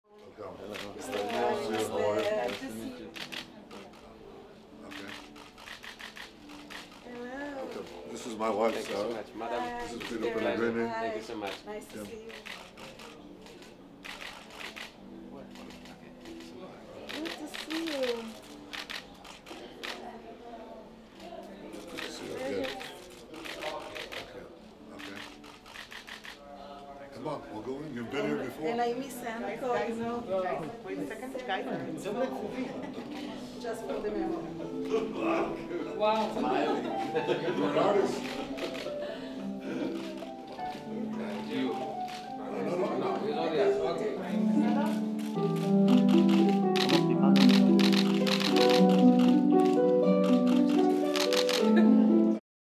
רה"מ נתניהו אירח את ר"מ הונגריה אורבן, ר"מ סלובקיה פלגריני ור"מ צ'כיה באביש ורעייתו
ראש הממשלה בנימין נתניהו ורעייתו הגב' שרה נתניהו אירחו את ראש ממשלת הונגריה ויקטור אורבן, ראש ממשלת סלובקיה פטר פלגריני וראש ממשלת צ'כיה אנדריי באביש ורעייתו לארוחת צהריים במעון רה"מ בירושלים.